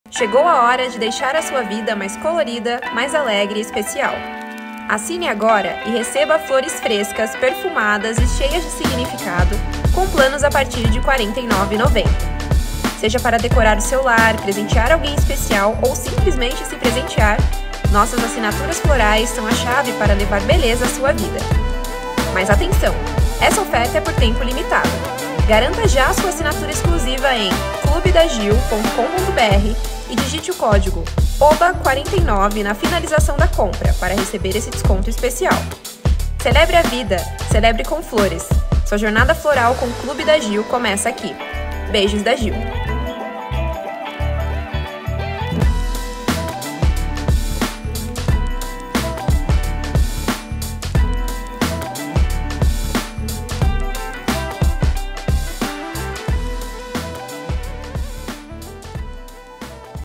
Conheça o Clube de Assinatura de Flores da Giuliana Flores | Clube da Giu
Conheça o Clube da Giu. O clube de assinatura de flores da Giuliana Flores. Receba na sua casa flores fresquinhas todo mês com a qualidade da Giuliana Flores. Aproveite a promoção de lançamento, use o cupom OBA49 e pague apenas R$49,90 nas duas primeiras mensalidades. Confira condições no site. Acesse agora mesmo. Pensou em assinatura de flores, pensou no Clube da Giu.